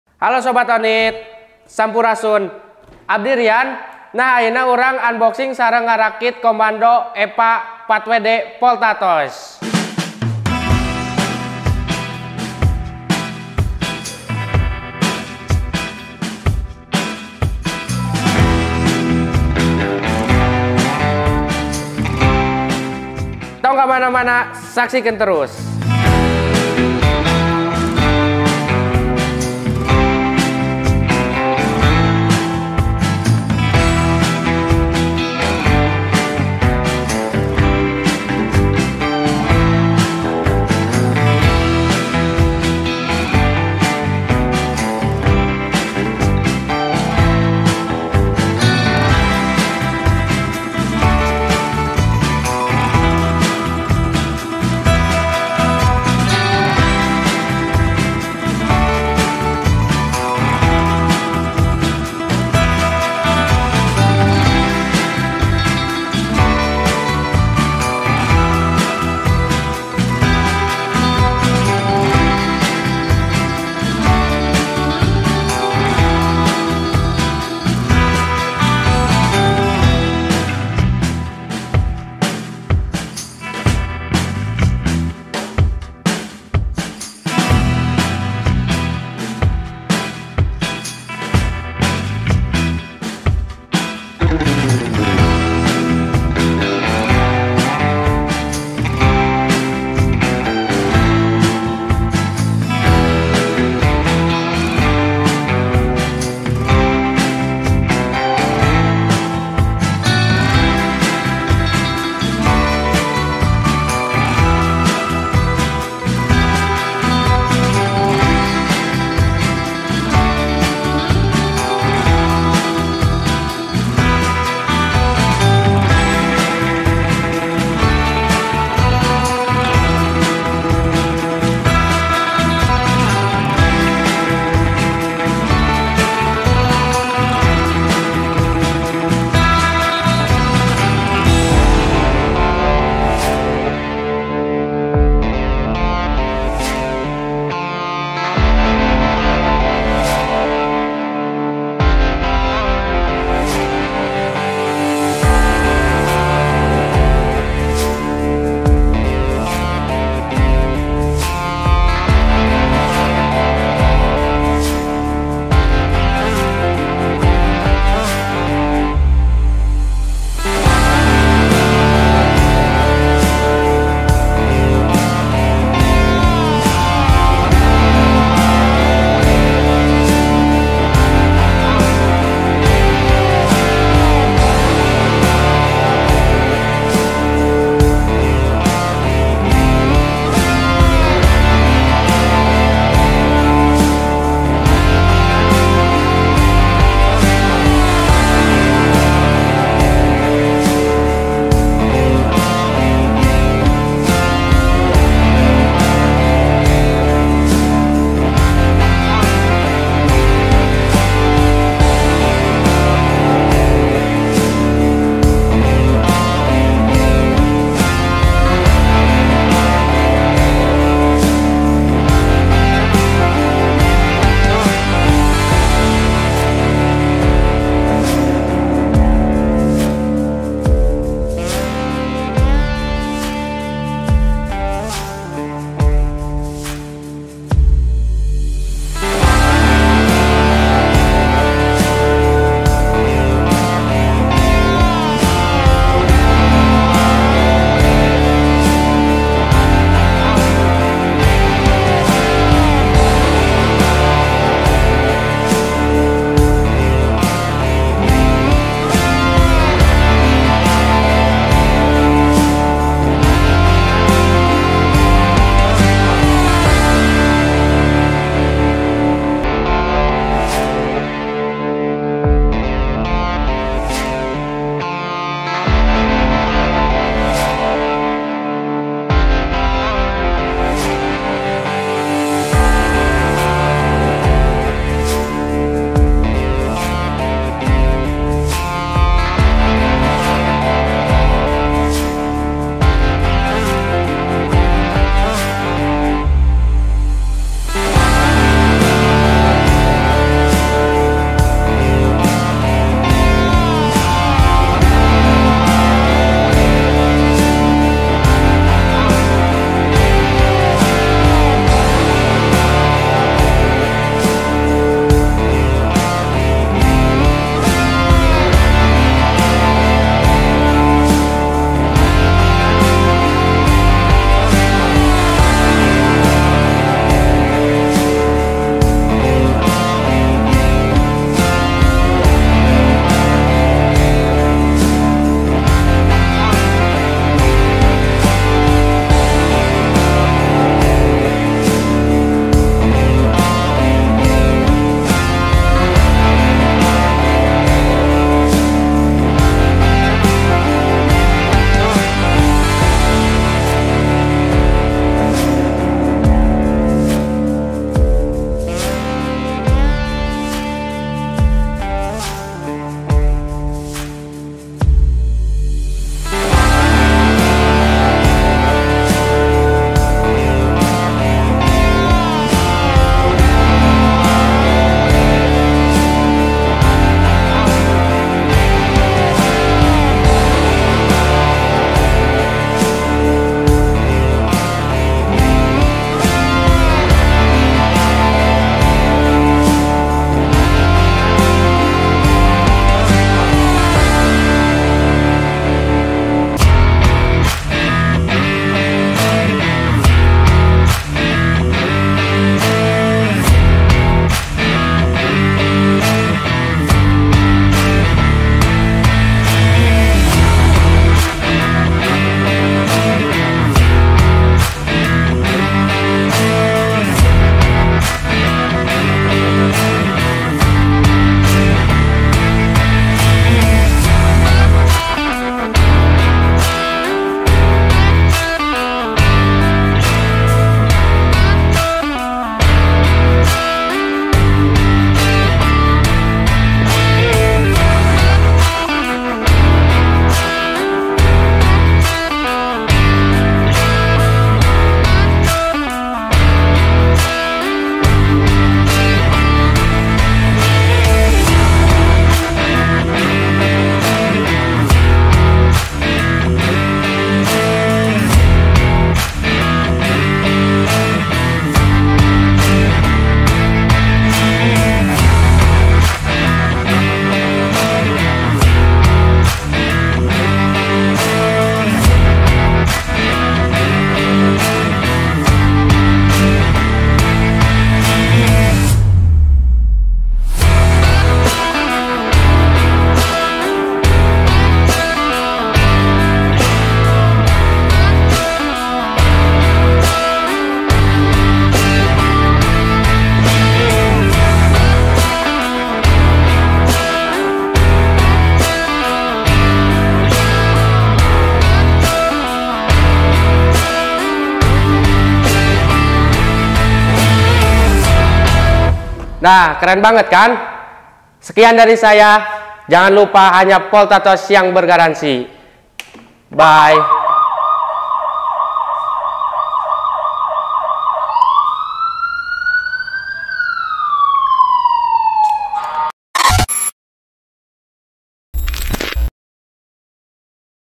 Unboxing & Merakit Mobil Aki Commando (Eva Wheels) Volta Toys - Volta 5088
Koleksi mainan terbaru dari SHP Toys Volta 5088 kini hadir dengan berbagai fitur baru dan lebih canggih yang akan membuat jiwa petualangan anak semakin seru saat bermain!!

Spesifikasi :
- Rotator with Handy Talky
- Daytime Running Light
- POWER 12V
- TORQUE 18.000 Rpm
- Twin Gearbox (Double)
- MP3 with USB Bluetooth
- 2 Point Seatbelt
- Slow Start (Anti Kejut)
- Smart Control (Android & ios)
- 2.4 Ghz Remote Control